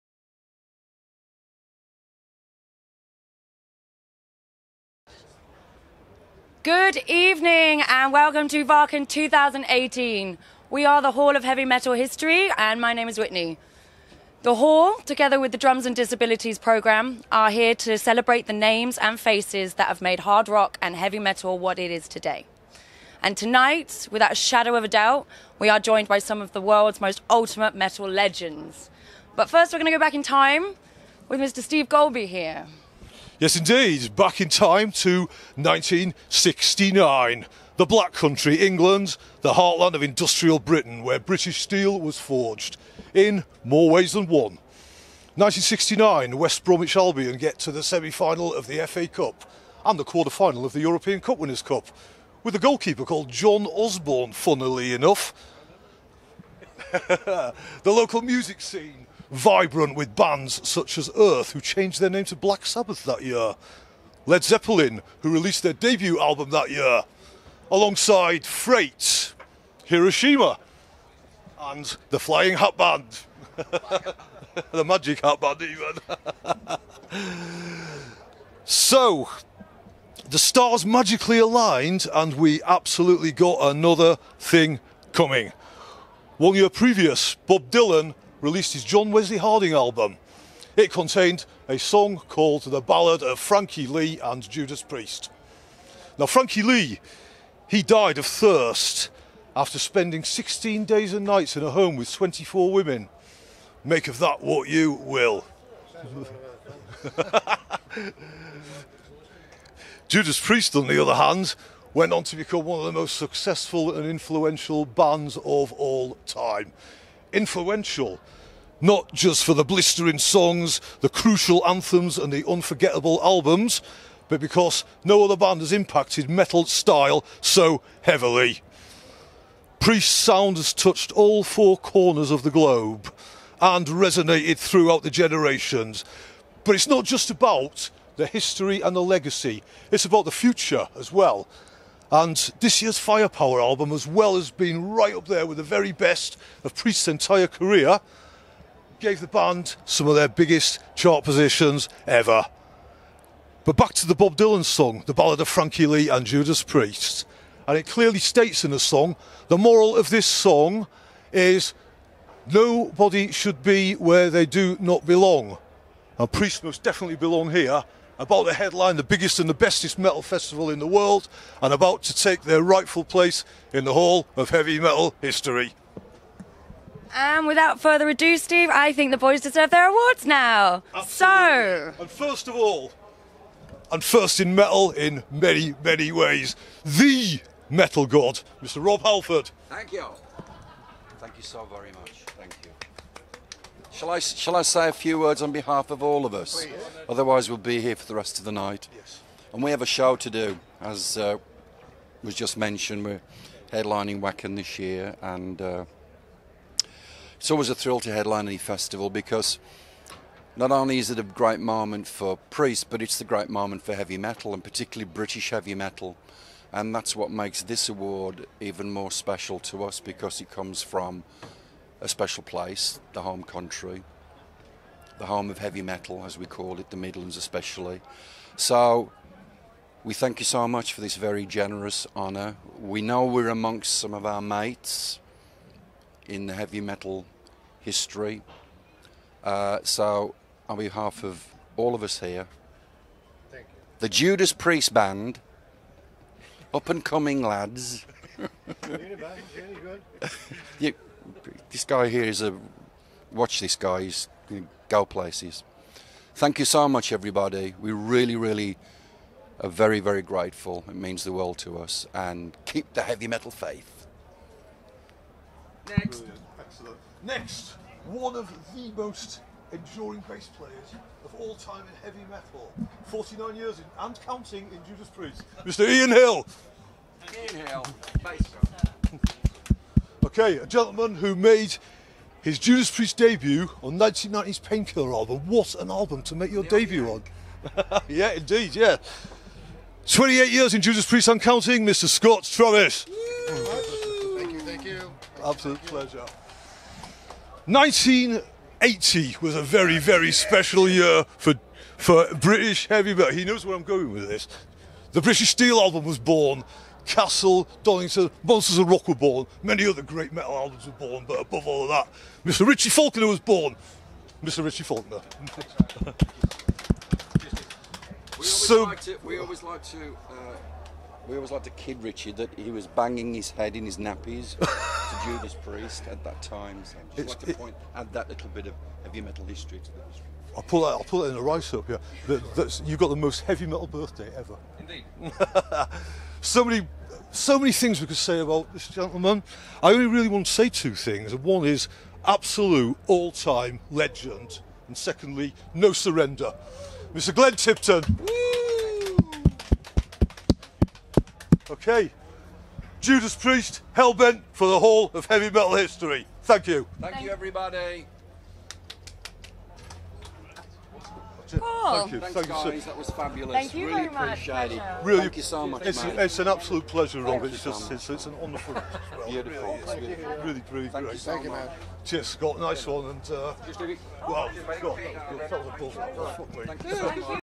Judas Priest Hall Of Heavy Metal History Induction
Judas Priest Hall were inducted into the Hall Of Heavy Metal History at Wacken Open Air on Thursday 2nd August 2018 by Whitney Alyson Ribbins and Steve Göldby. Here's the complete ceremony.